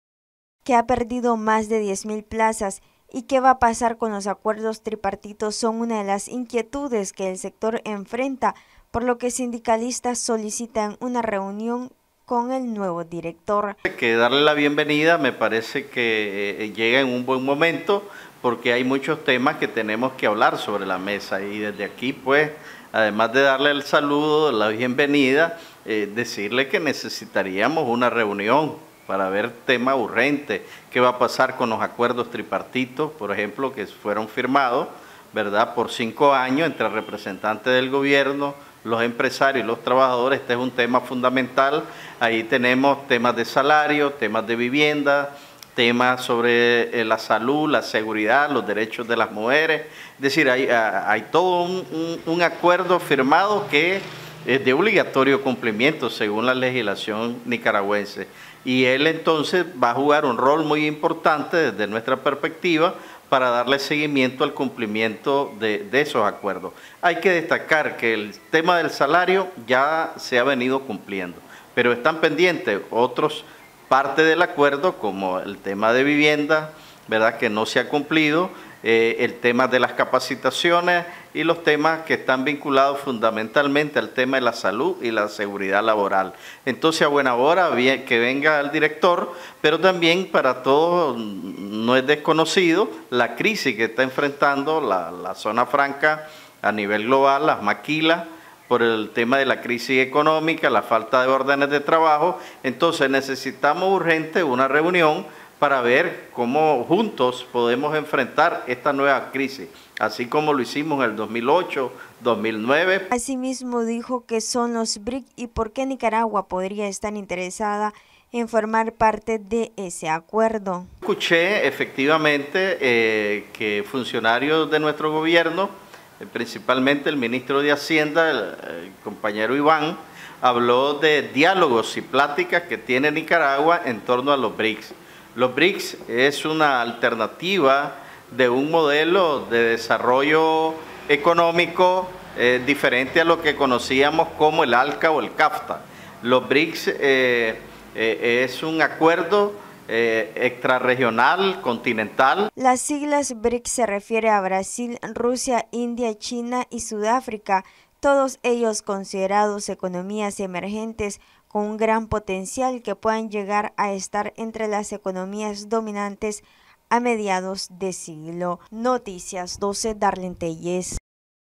Noticias12Nicaragua Sindicalistas solicitan reunión con nuevo director Ejecutivo de Zonas Francas
Sindicalistas solicitan reunión con nuevo director Ejecutivo
Interino de la Corporación de Zonas Francas
Ante el nombramiento de nuevo director Ejecutivo Interino de la Corporación de Zonas Francas, sindicalistas de ese sector solicitaron una reunión, para que aborden la situación que atraviesan las maquilas y qué va pasar con los acuerdos tripartitos